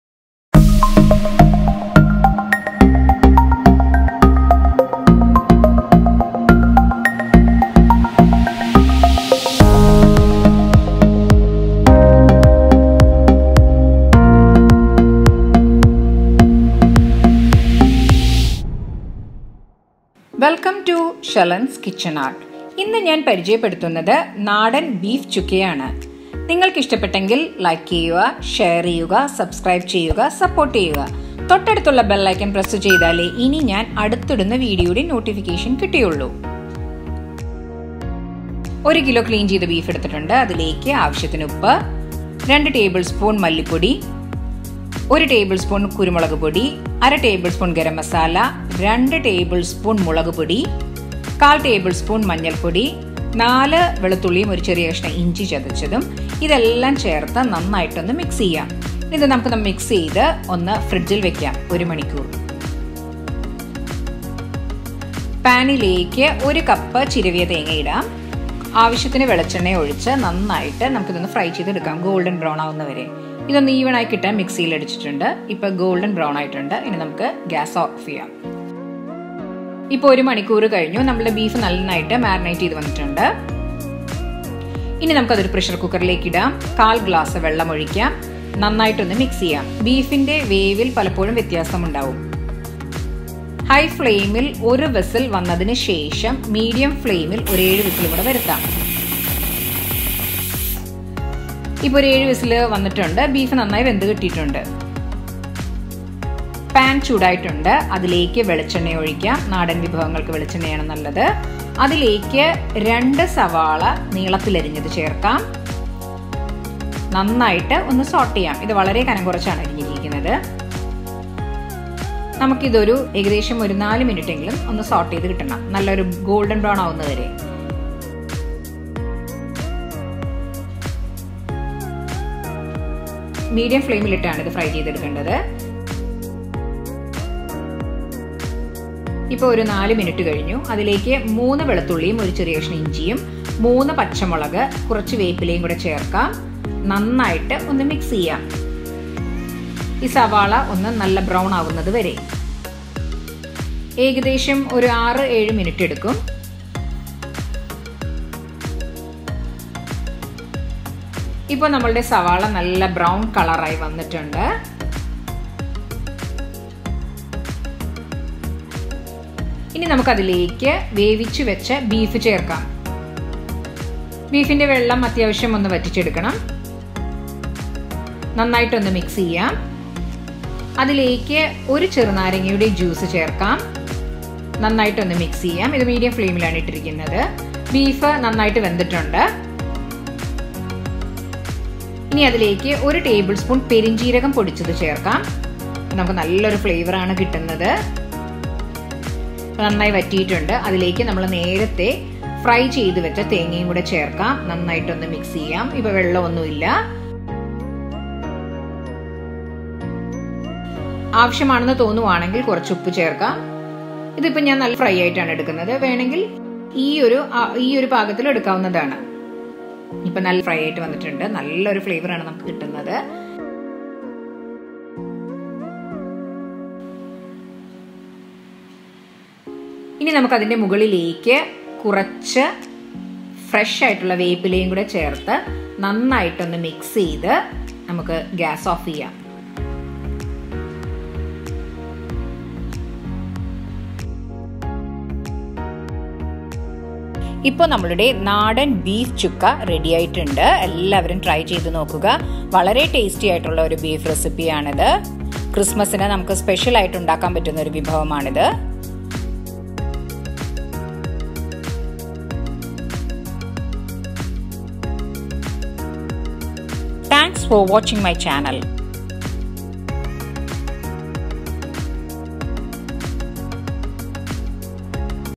Naadan Beef Chukka | നാടൻ ബീഫ് ചുക്ക | By Shalin's Kitchen Art
Ingredients

Beef-1 kg
Chilly powder-2 tablespoon
Pepper powder-1 tablespoon
Onion-2
Green chilly-3
Garlic -7
Ginger-2 small piece
Lemon juice-1
Grated coconut-1 cup
Fennel seeds-1 spoon
Lemon juice-1
Little curry leaves
Coconut oil as required

In a pan add 1 kg beef, 2 spoon chilly powder,1 spoon coriander powder, 1 spoon pepper powder, 1/2 spoon Garam masala, 1/4 spoon turmeric powder, 4 garlic and 1 small piece ginger crushed, salt as required mix well and marinate it for 1 hour in refrigerator. In a pressure cooker add these ingredients and add 1/4 glass water to it mix well make 1 whistle in high flame and 6-7 whistles in low flame. In a pan add 1 cup grated coconut, add little coconut oil and fry it until golden brown. In a pan add coconut oil, add 2 chopped onion sauté it well until it turns golden brown, add 3 chopped green chilly, 3 garlic and 1 small piece ginger crushed, little curry leaves saute well add the cooked beef sauté well then add the fried coconut and 1 spoon crushed fennel seeds and 1 lemon juice mix well and fry it until golden brown. our Beef chukka is ready.